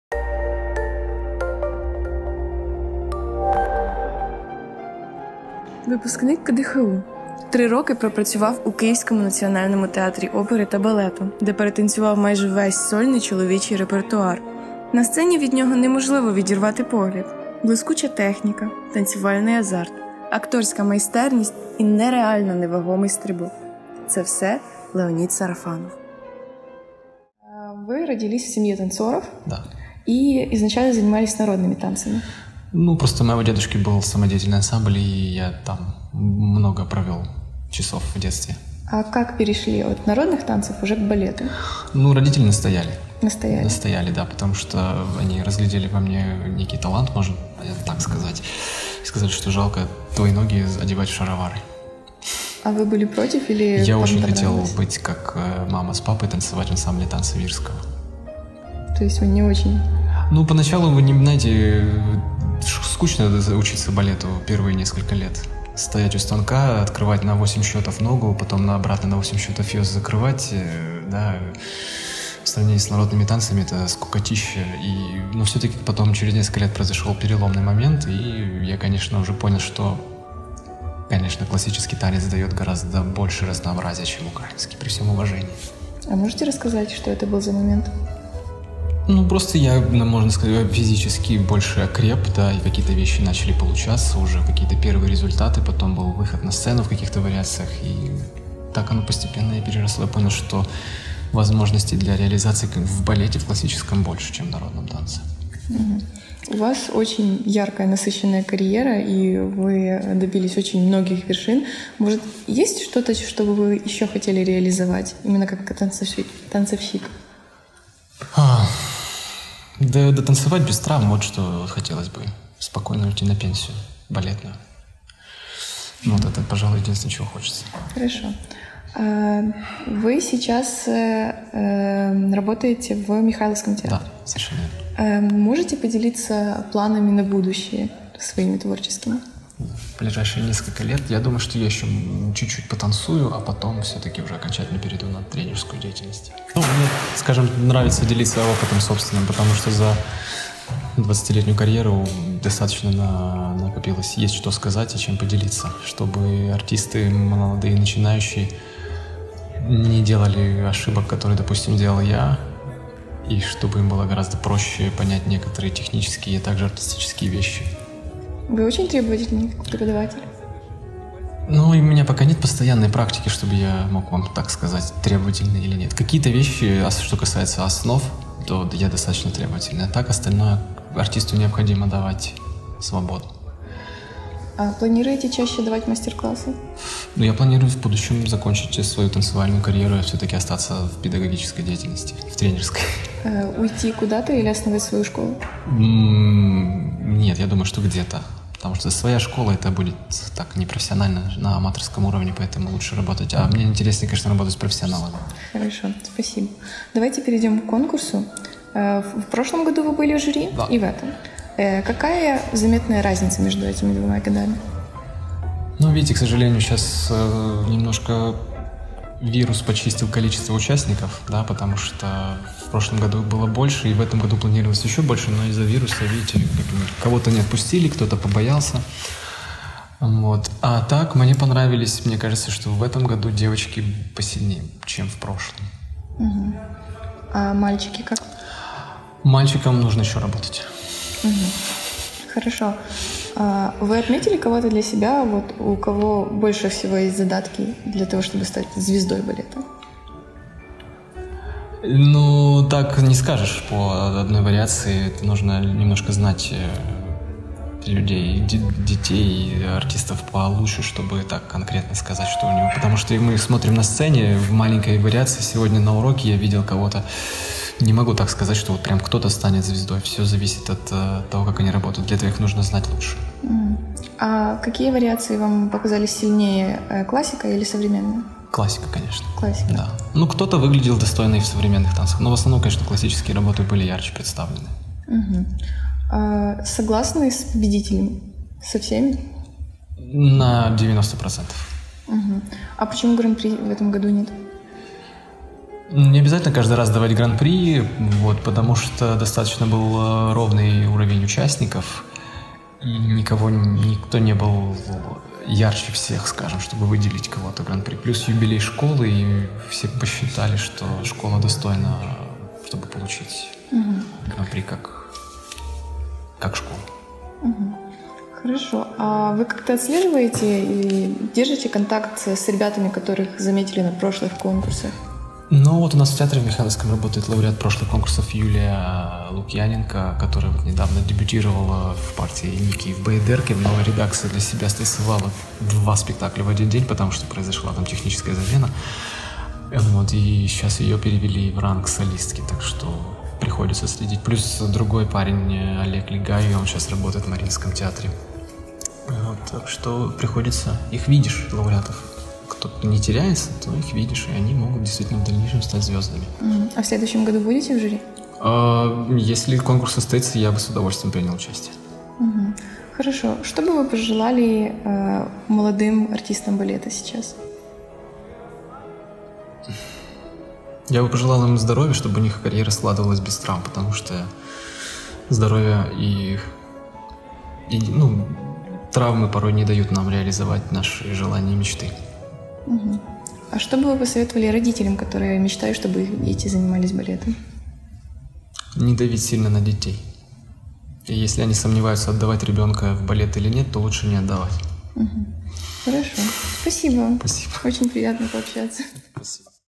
ЛЕОНІД САРАФАНОВ | GRAND PRIX KYIV | UWCF TALKS
Український танцівник балету родом з Києва, Леонід Сарафанов - про різницю між класичним танцем та українським народним, про перспективи на майбутнє та молодих українських артистів - і не тільки.

THANKS OUR DOP, EDITOR, DIRECTOR: DANYLA KINZERA